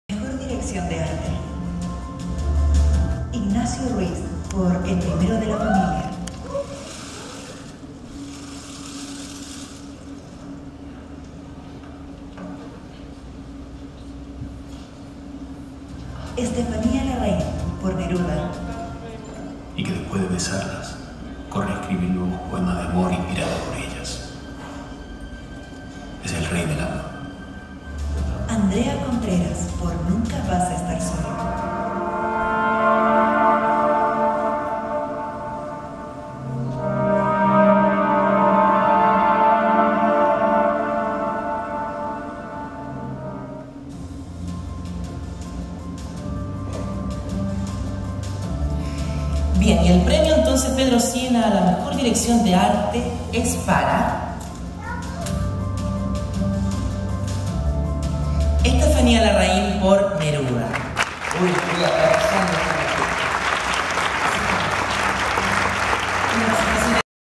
Premios Pedro Sienna 2017 - Vídeo 24
5 de agosto 2017.- La 11° versión de los Premios Pedro Sienna de cine chileno tuvo lugar en el Teatro Municipal de Chillán. Fotos en Facebook Intercomuna Chillán Chillán Viejo.